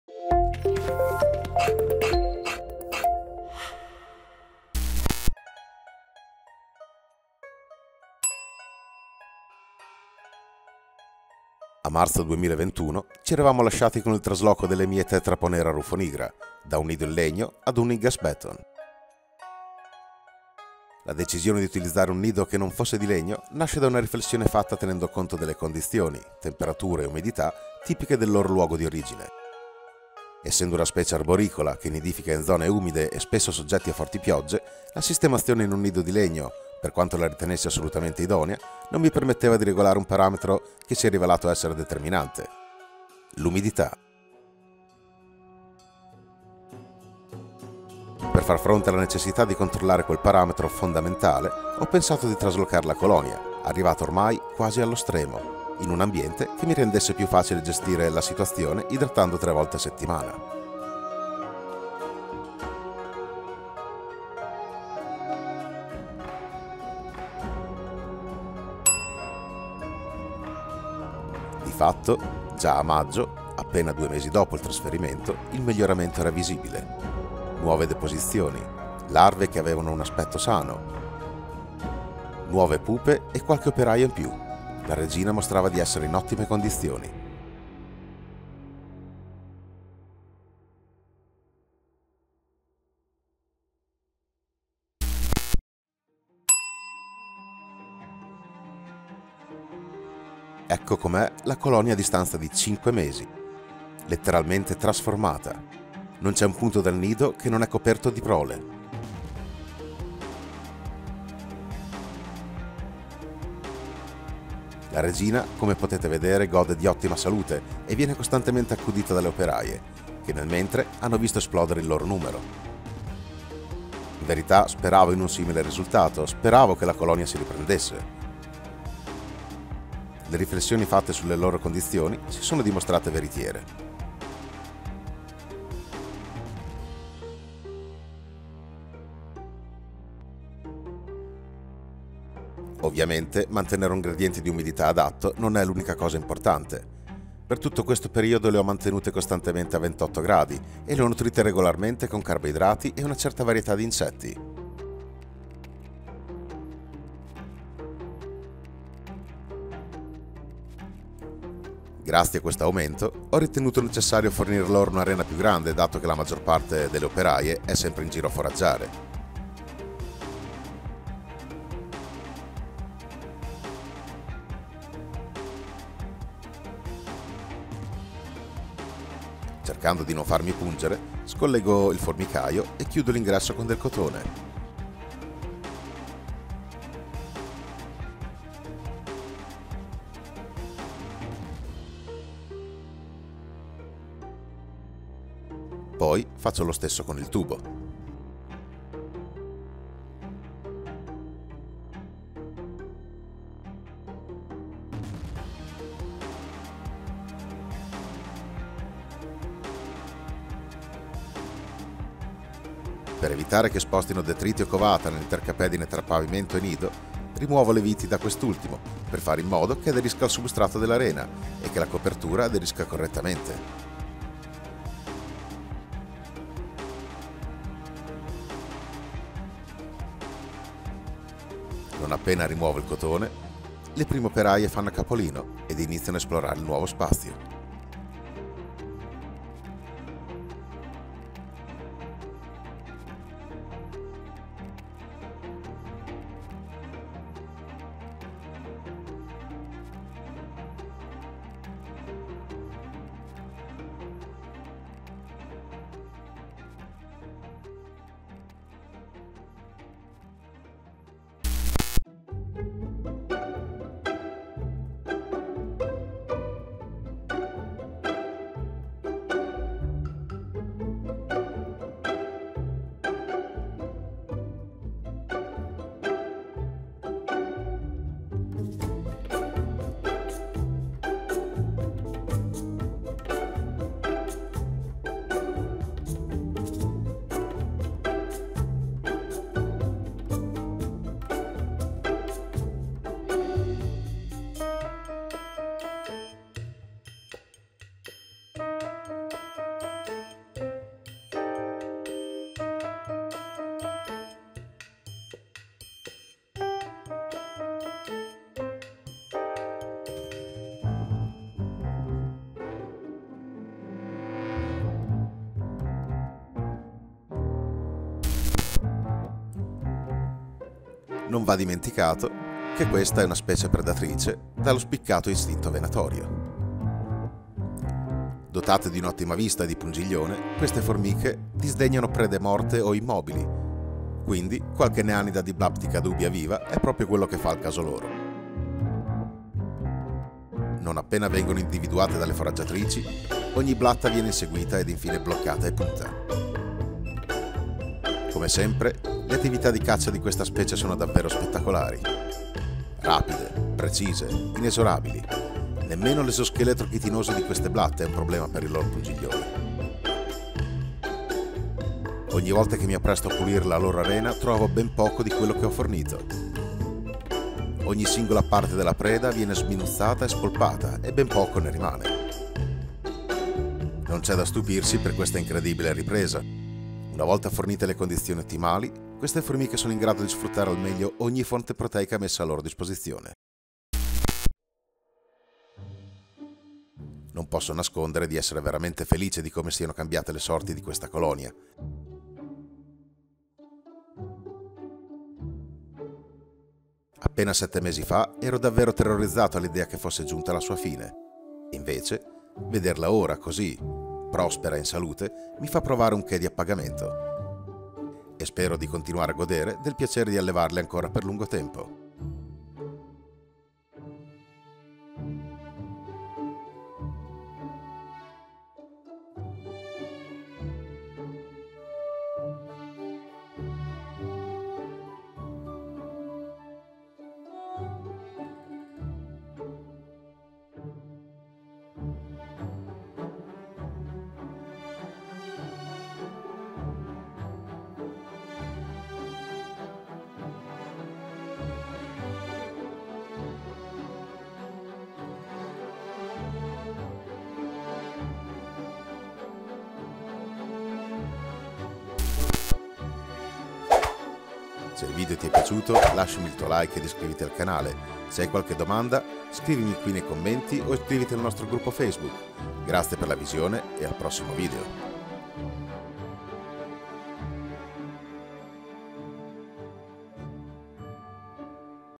Come ho Salvato la mia Colonia di Formiche Grazie ad un Cambiamento di Nido (EP.6)
*Diario 6* - _La decisione di utilizzare un nido che non fosse di legno, nasce da una riflessione fatta tenendo conto delle condizioni , temperatura e umidità, tipiche del loro luogo di origine. Essendo una specie arboricola che  nidifica in zone umide e spesso soggette a forti piogge, la sistemazione in un nido di legno, per quanto la ritenessi assolutamente idonea, non mi permetteva di regolare un parametro che si è rivelato essere determinante. L’umidità..._

Per altri consigli , suggerimenti, foto ed immagini sul mondo delle formiche entra anche nel gruppo Facebook di ANTS ITALIA!
Segui anche la nostra pagina Instagram!

Si ringraziano @leformichedelgianlu4793 ,F. Ricatti, R. Piras e  M.Green per l'importante contributo alla realizzazione di questo video.